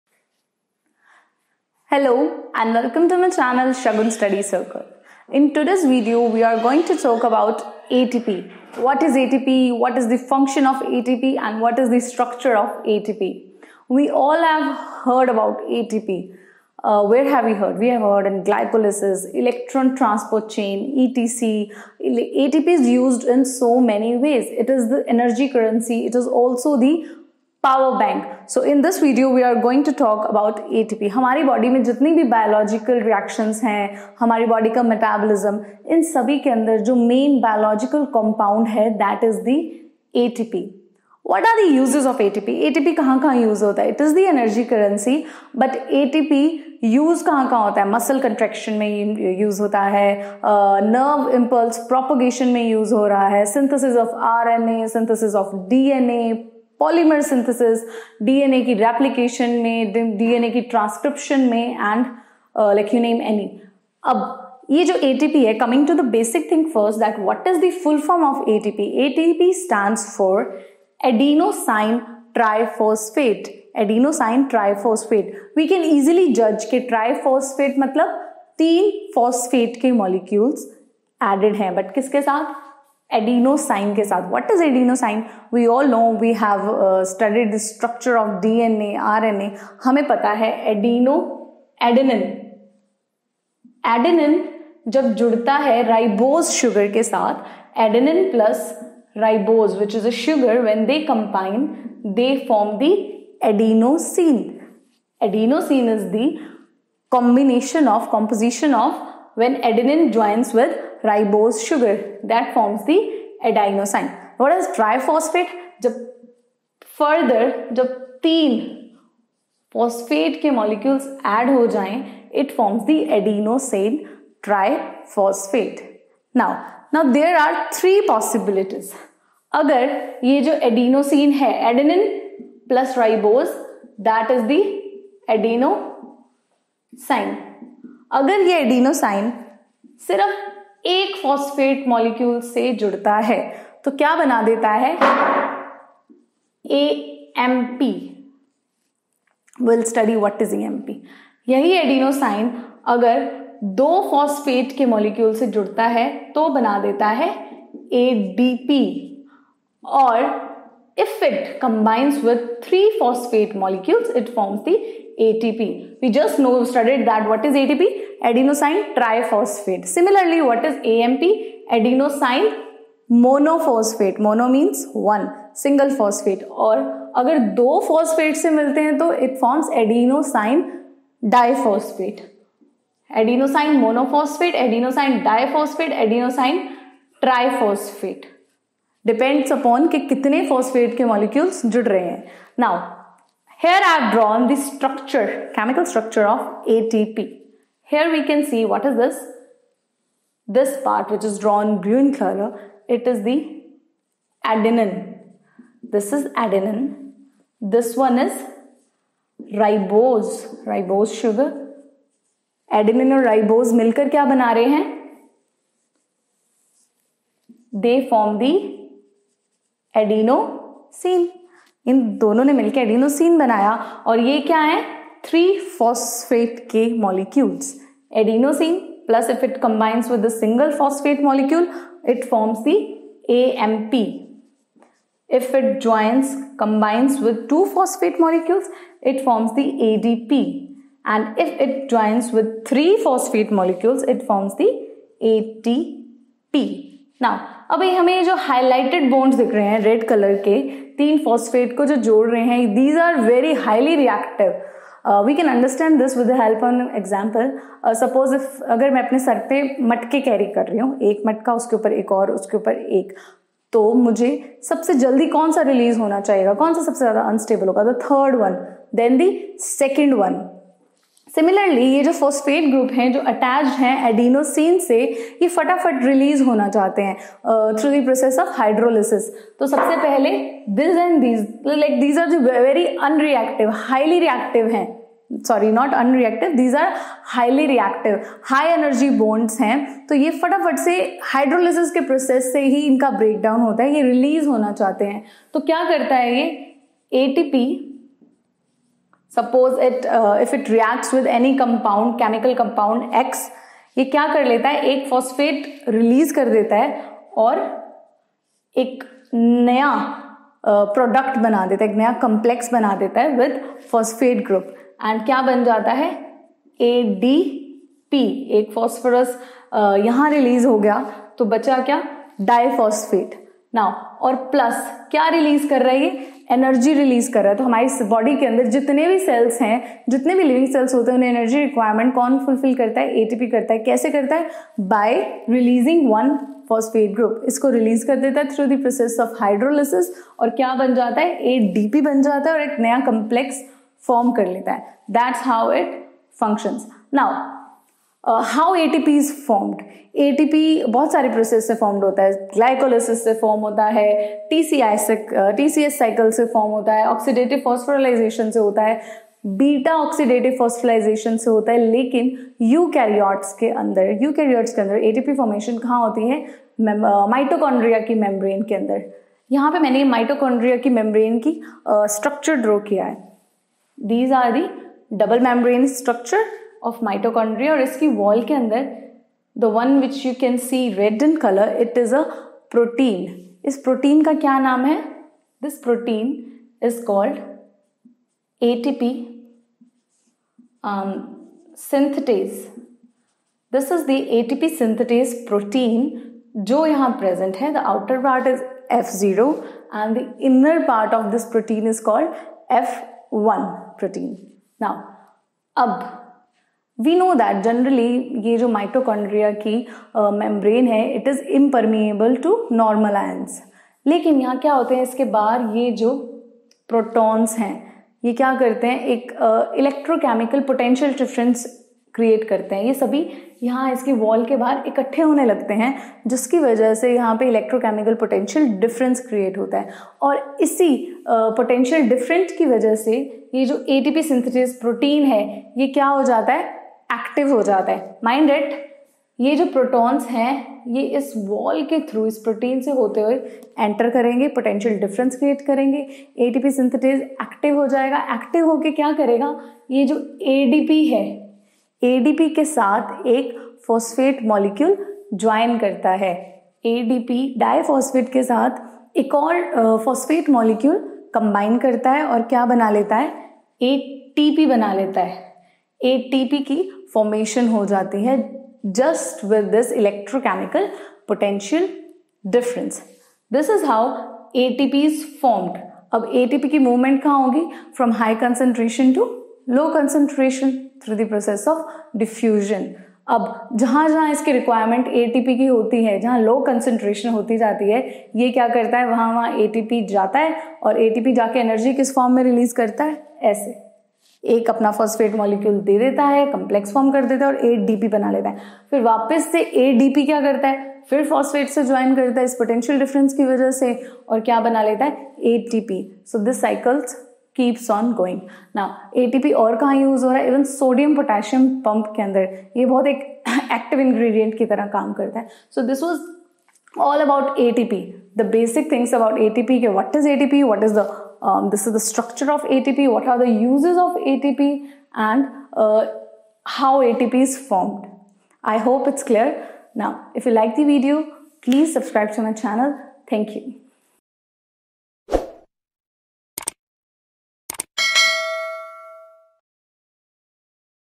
What is ATP/Adenosine Triphosphate/Structure and Function of ATP/Energy Currency/Shagun Study Circle
The human body is a complicated entity that requires energy to function properly. At the cellular level, adenosine triphosphate (ATP) is the energy source for use and storage. Adenosine triphosphate (ATP) is a nucleoside triphosphate with three serially linked phosphate groups and a nitrogenous base (adenine). ATP is known as the "energy currency" of the body.
The hydrolysis of ATP supports a variety of cell activities, including signalling and DNA/RNA synthesis, in addition to generating energy. Energy for ATP production comes from a variety of catabolic pathways, including cellular respiration, beta-oxidation, and ketosis. The majority of ATP is synthesised in the mitochondrial matrix during cellular respiration.